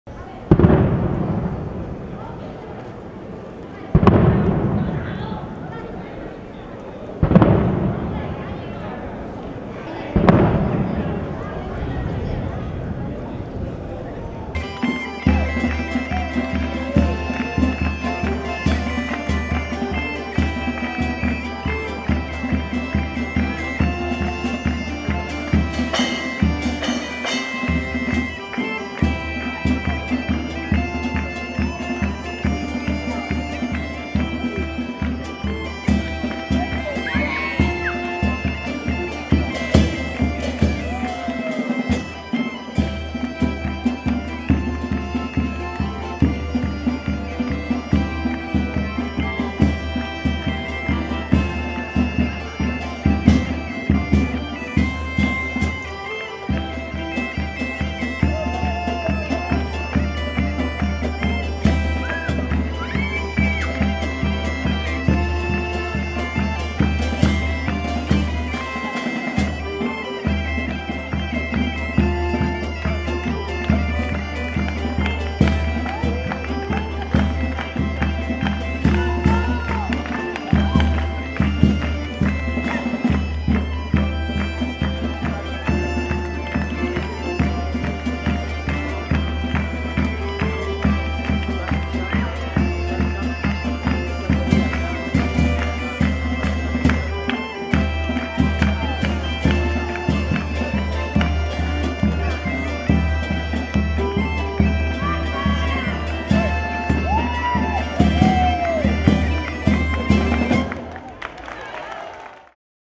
Arrancan as Festas da Ascensión
Coas tradicionais bombas de palenque e os cabezudos deuse o pistoletazo de saída as Festas da Ascensión 2019